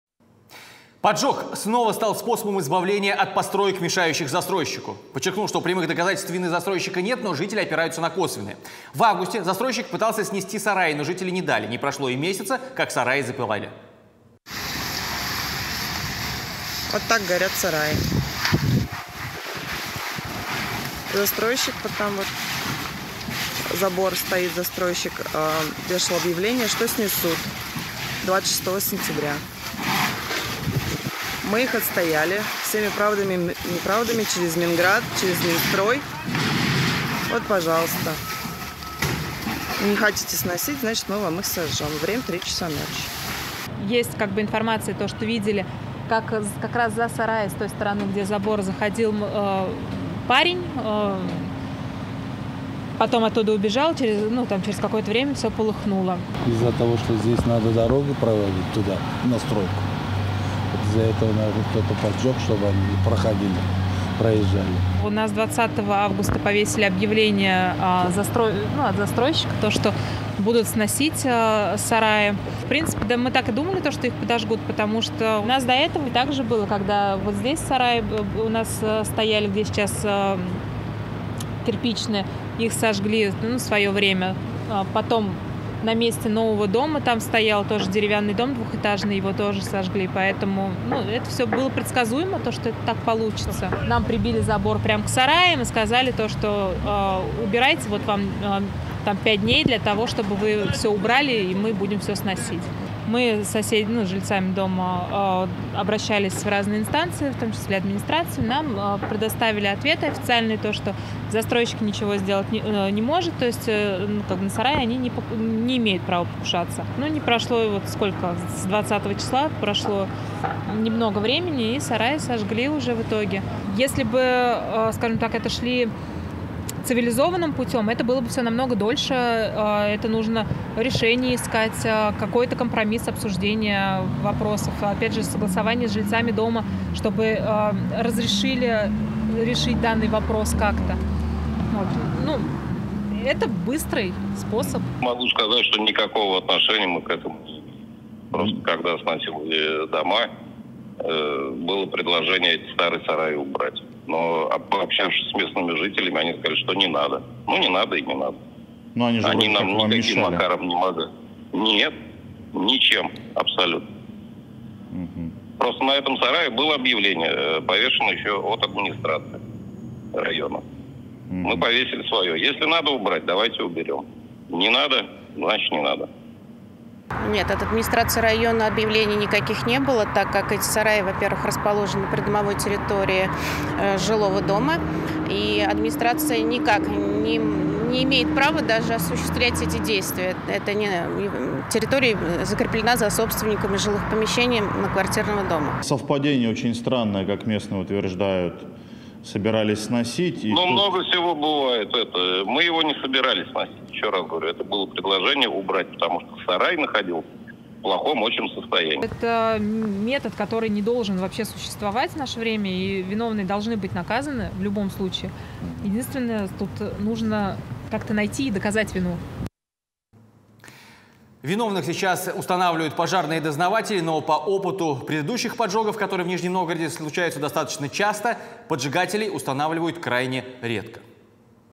После того, как застройщику не дали снести сараи, они были сожжены
Поджог снова стал способом избавления от построек мешающих застройщику. Подчеркну, что прямых доказательств вины застройщика нет, но жители опираются на косвенные. В августе застройщик пытался снести сараи, но жители не дали. Не прошло и месяца, как сараи запылали.